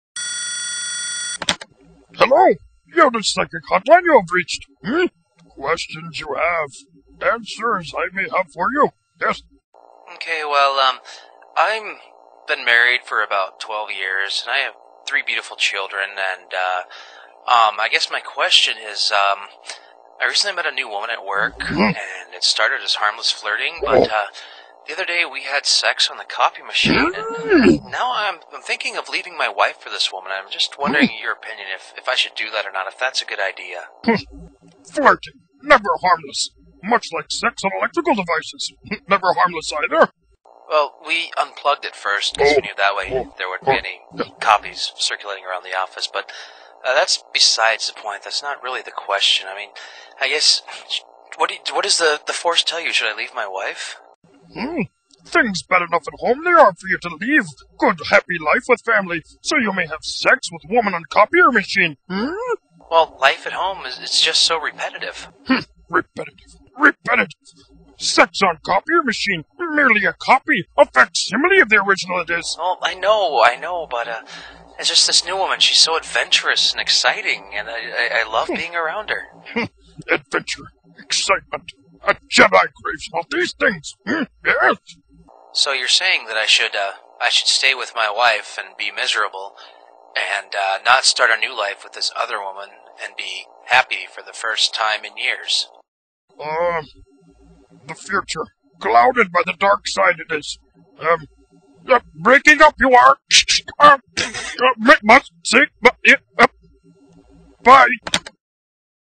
Yoda's Psychic Hotline
Yoda tries his hand at running a psychic hotline.